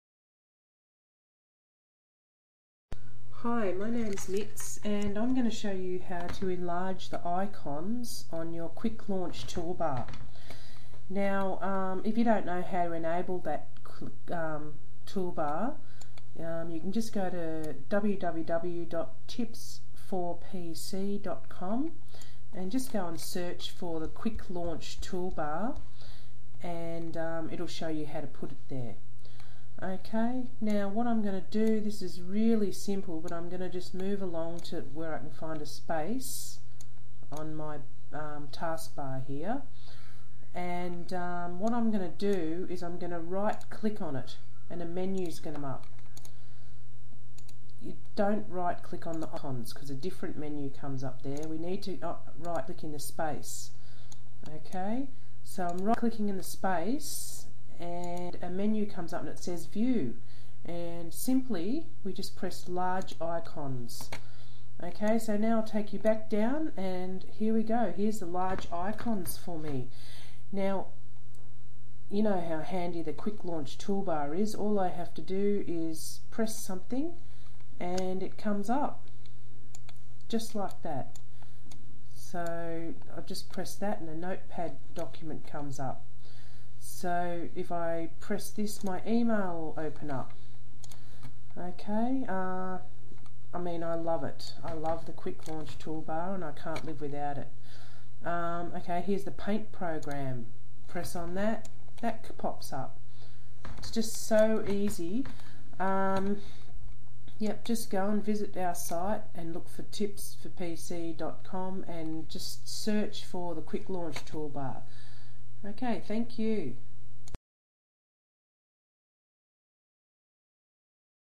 Enlarge icons on your quick launch toolbar in Windows XP
Make the icons bigger on your quick launch toolbar.